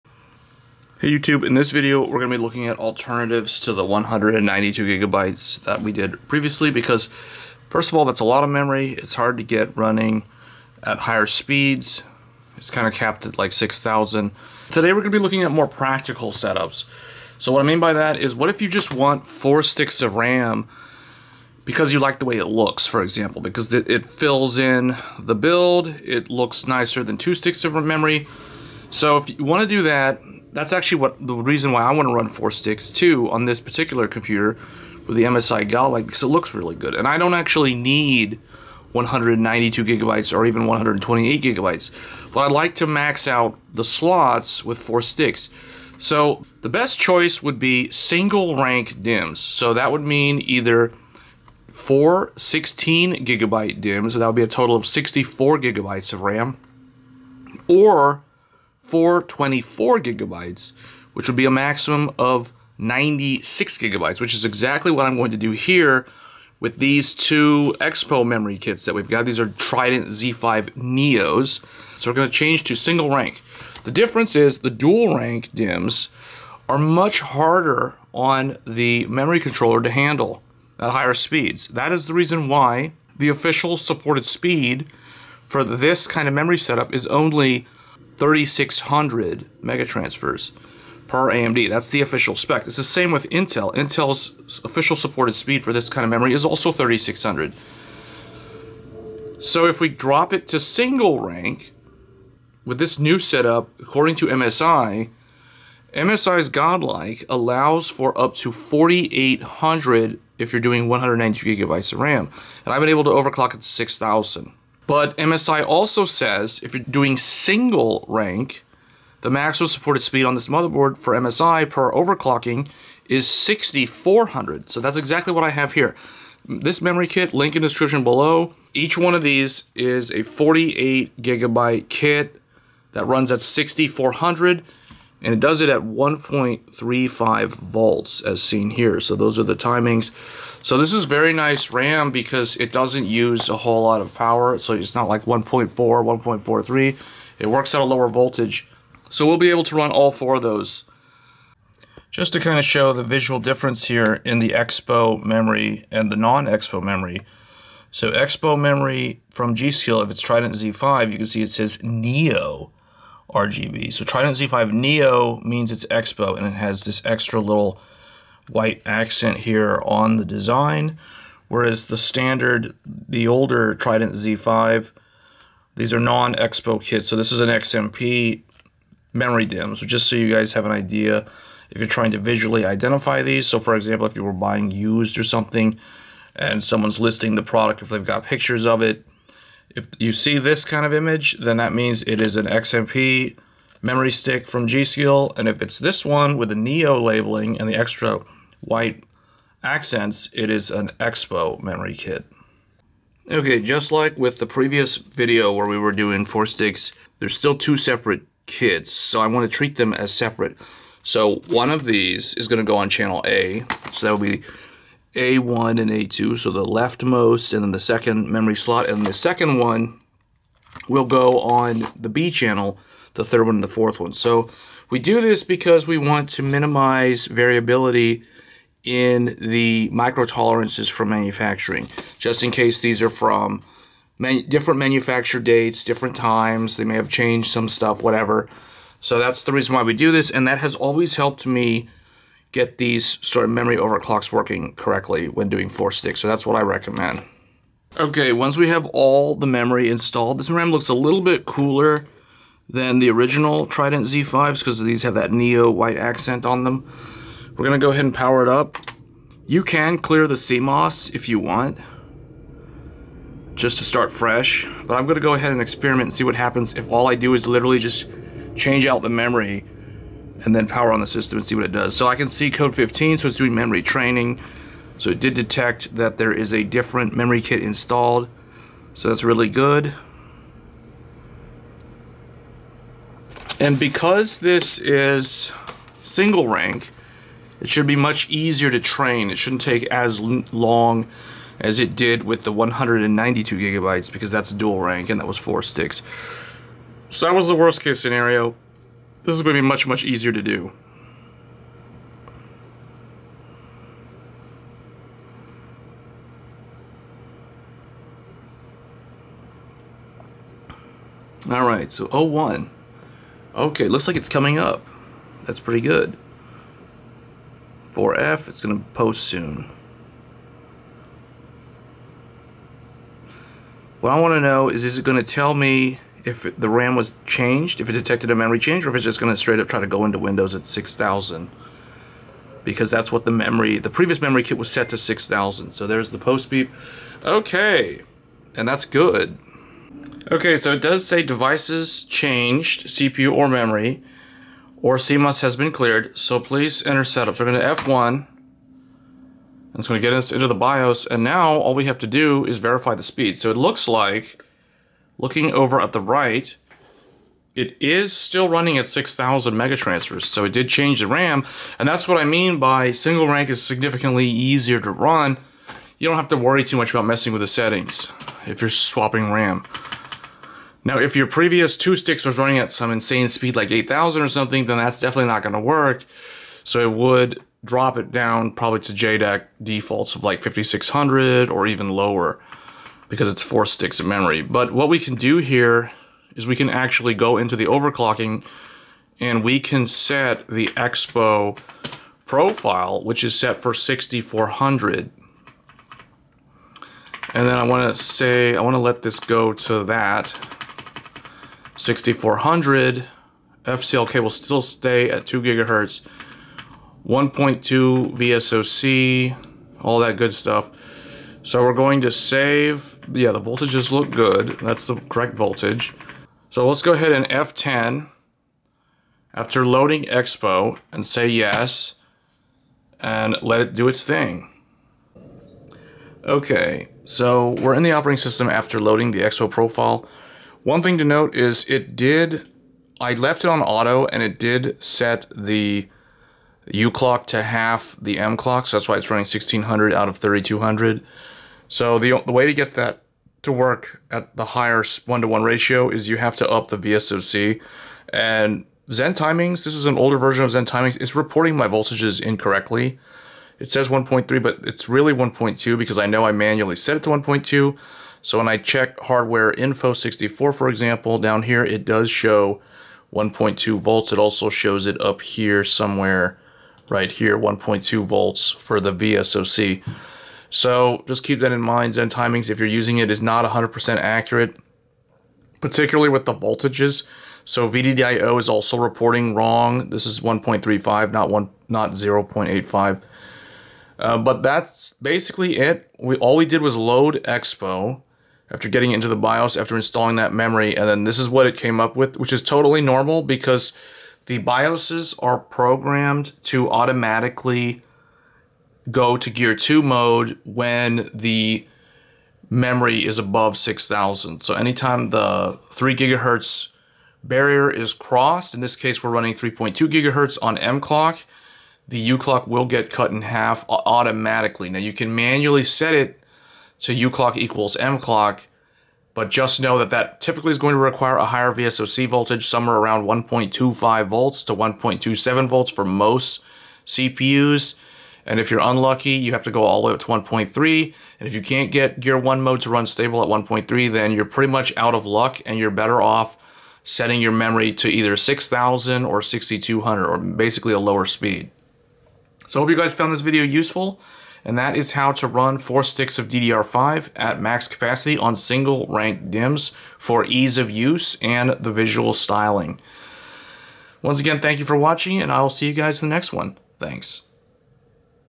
How to run 4 sticks of DDR5 6400 | X870E Godlike | 96GB
This video is for those who want to run 4 sticks of DDR5 because it looks better than 2 sticks.

Recommended DDR5 for 4x24gb (use 2 of this kit)

Timestamps:
0:00 - Intro
2:32 - Trident Z5 NEO vs Trident Z5
4:08 - Initial boot
5:43 - Enable EXPO in bios
7:05 - Verify memory settings in OS